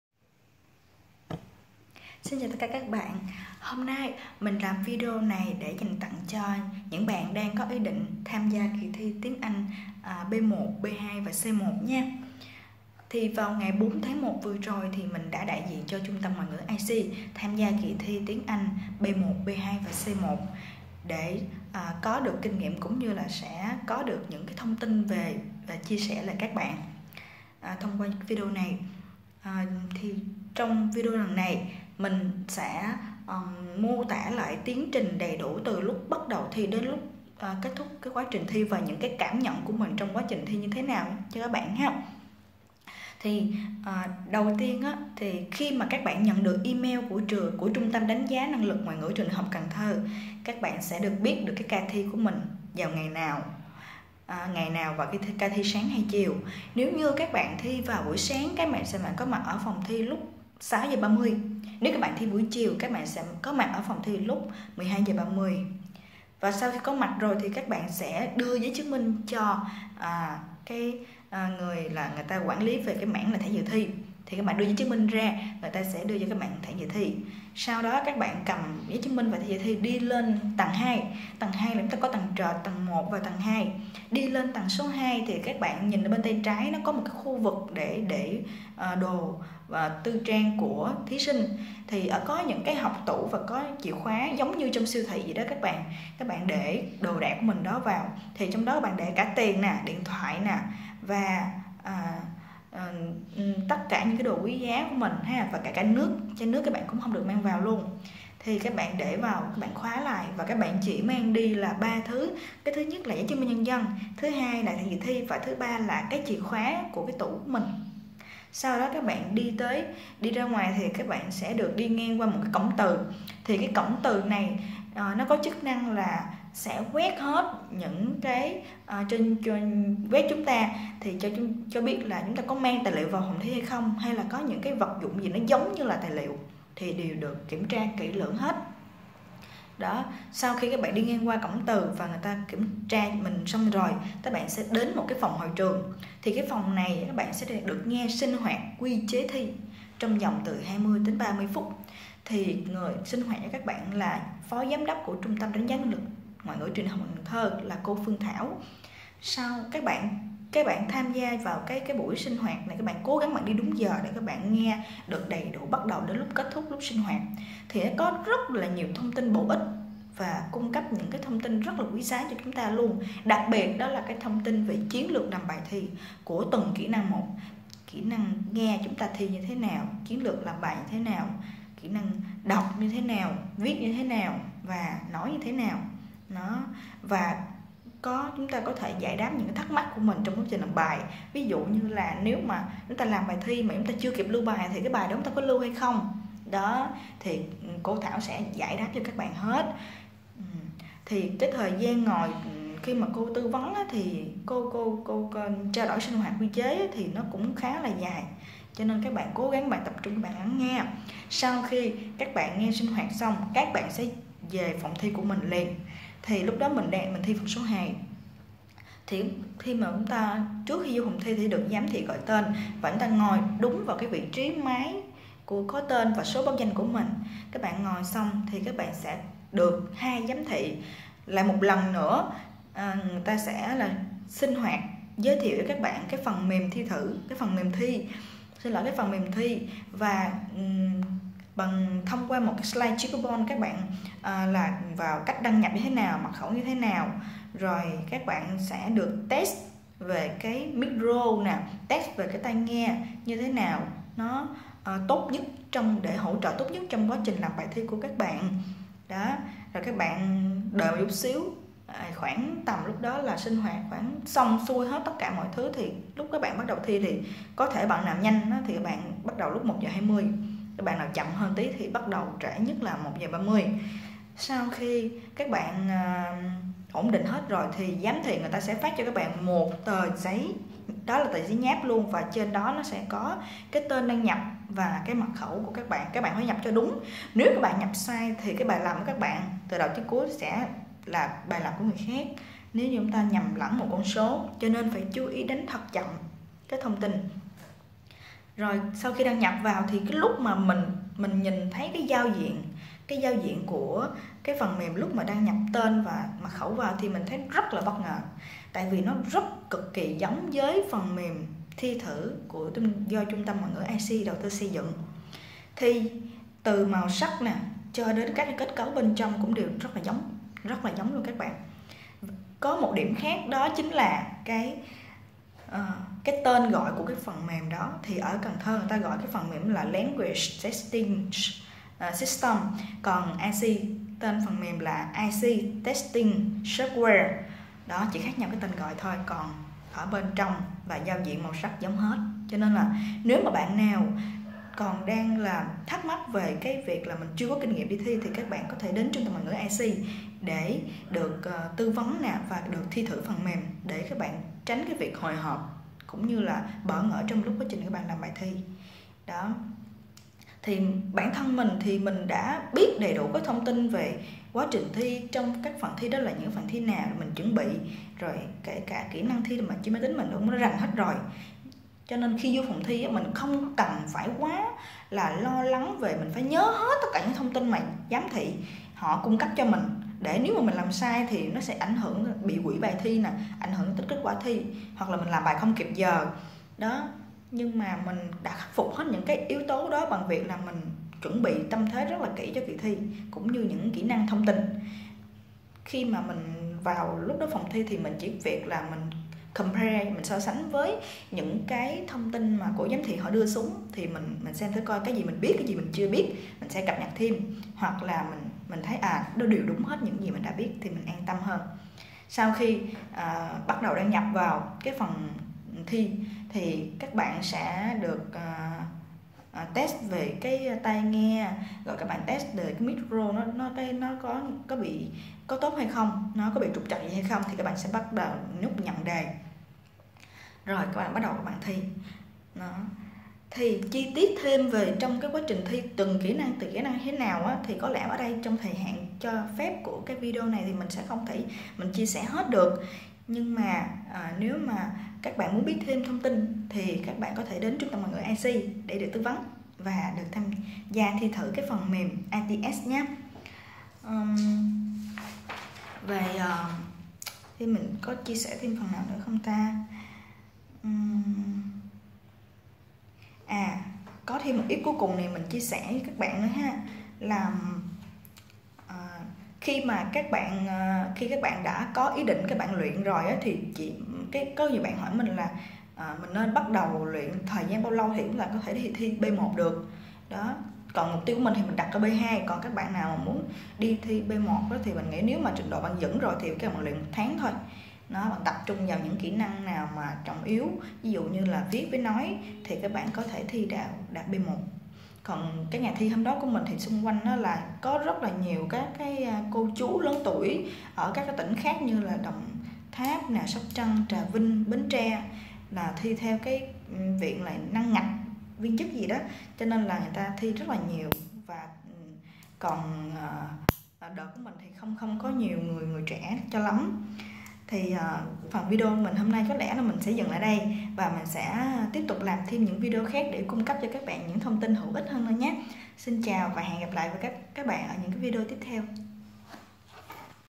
Review về kì thi Tiếng Anh B1-B2-C1 ngày 04/01/20 tại TT Đánh giá năng lực Ngoại ngữ trường ĐHCT
Nhằm giúp thí sinh có thêm kinh nghiệm về kì thi Tiếng Anh 6 bậc của Bộ GD & ĐT, Trung tâm ngoại ngữ iSEE mô tả chi tiết tiến trình bắt đầu đến lúc kết thúc của kì thi trên máy tính với nhiều điều còn mới mẻ tại Trung tâm Đánh giá Năng lực ngoại ngữ trường Đại học Cần Thơ.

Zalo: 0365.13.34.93
Địa chỉ: 26-27, D2, Đinh Trường Sanh, Đông Xuyên, Long Xuyên, An Giang (Gần công viên trường ĐH An Giang)